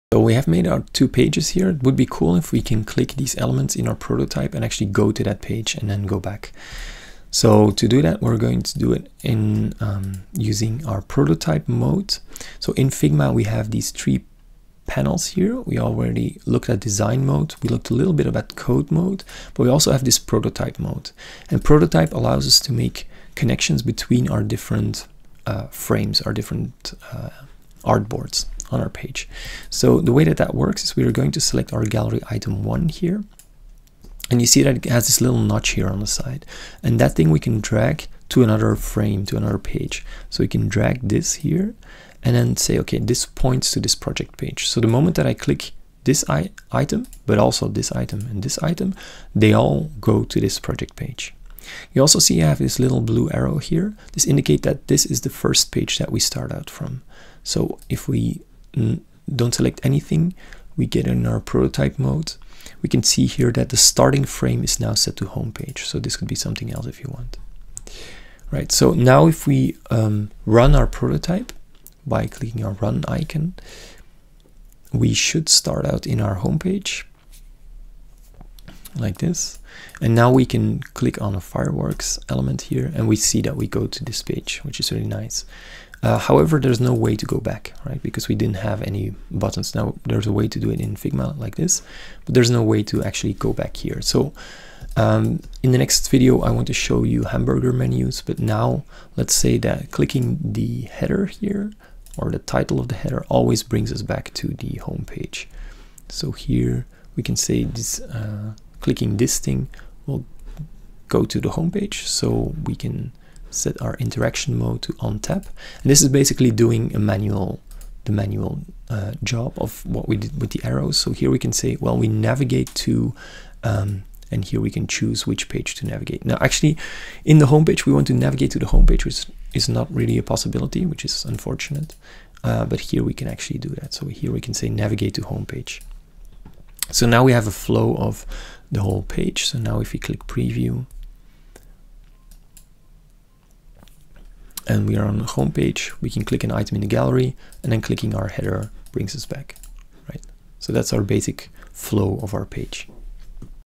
Getting Started with Figma: Connecting Screens Together
To navigate between screens, we'll use Figma's Prototype mode.